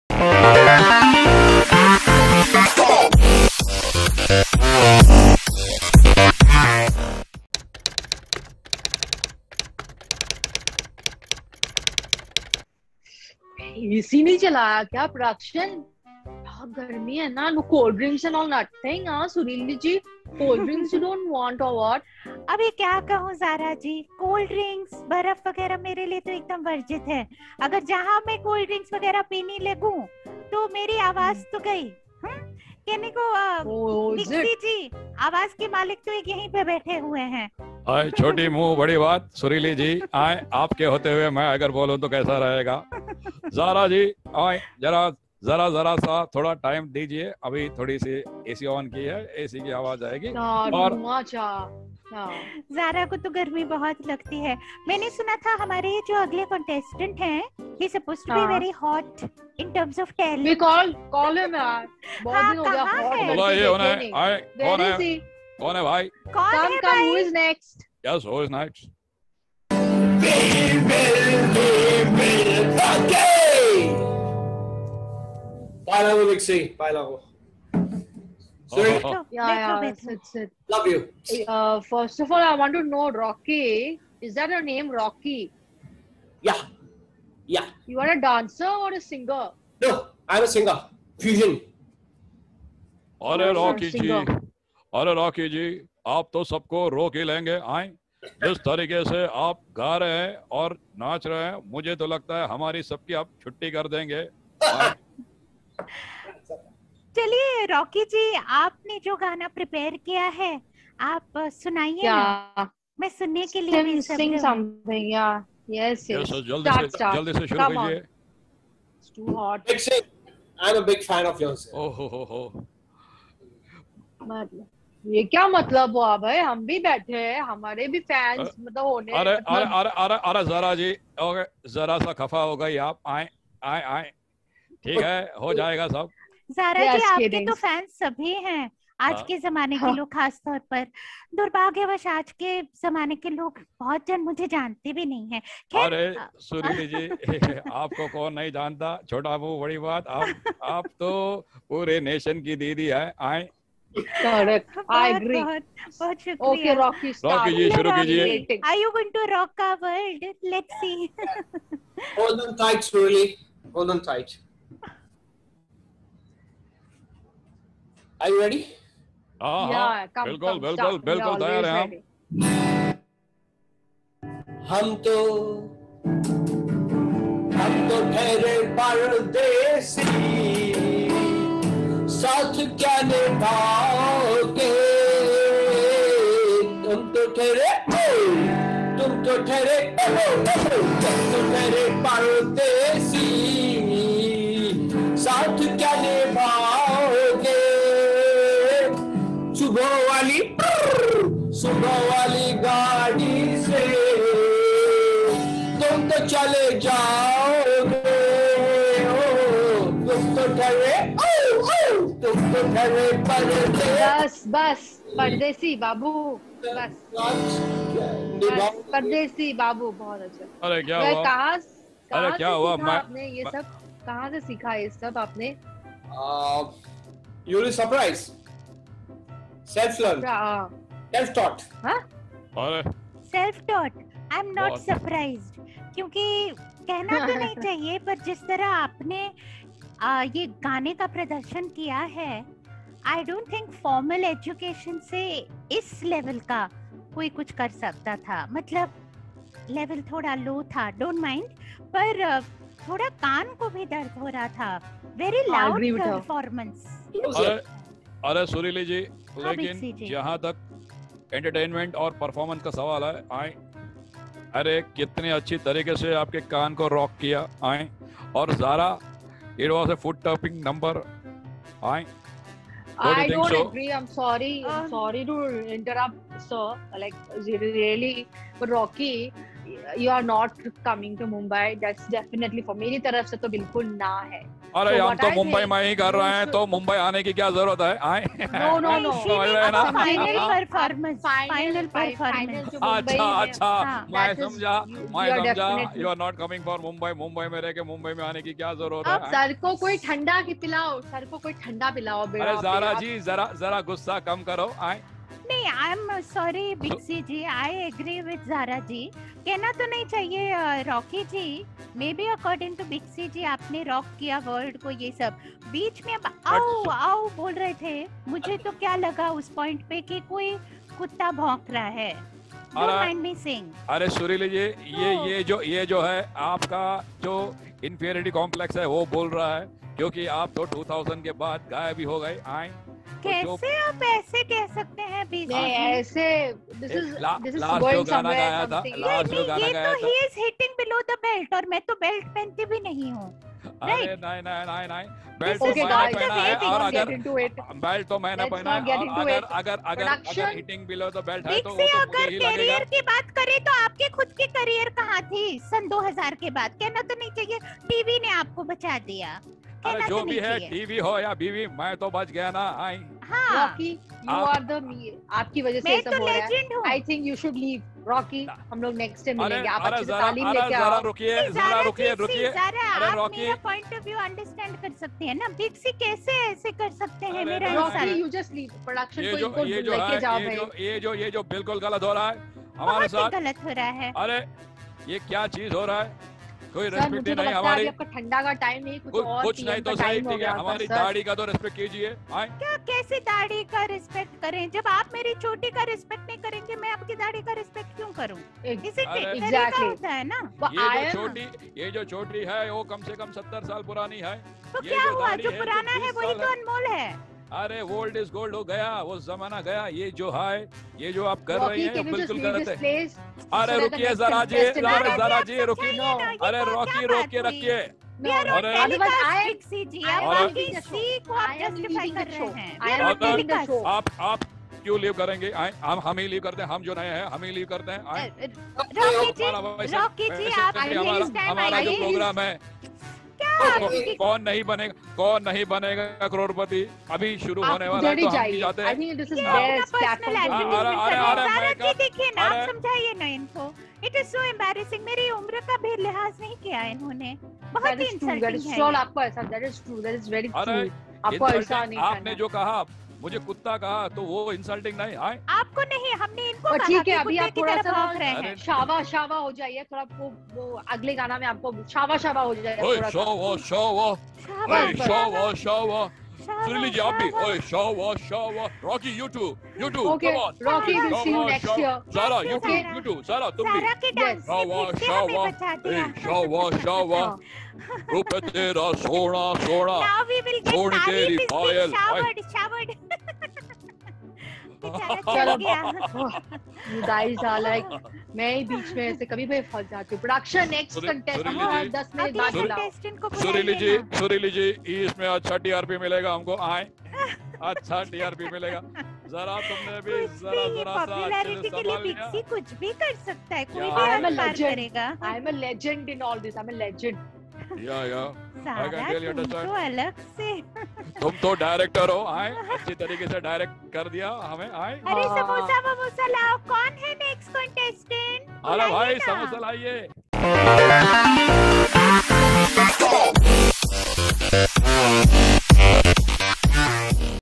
Improv Idol : Contestant Rocky
This time on Improv Idol fusion "feku" artist Rocky tries his best to impress judges - megastar Big C, dance choreogrpaher Zara Khan and legendary singer, Sureeli Devi - but an unexpected turn of events steal his thunder - yenjoi :)